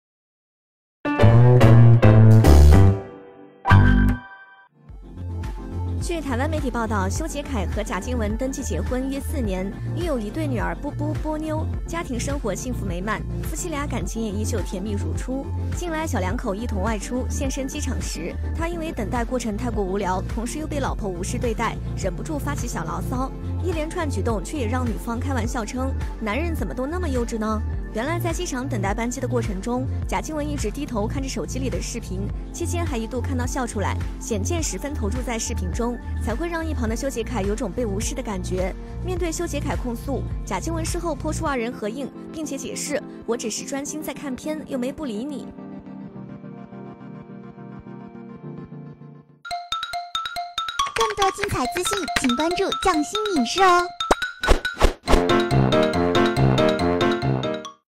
修杰楷自曝机场遭老婆“无视” 贾静雯发文解释原因
修杰楷和贾静雯结婚约4年，二人很是恩爱，近日二人去机场，贾静雯在玩手机，修杰楷录视频发牢骚，还被老婆无视，贾静雯看到后，发文回复，我只是在专心看片。二人真是日常晒狗粮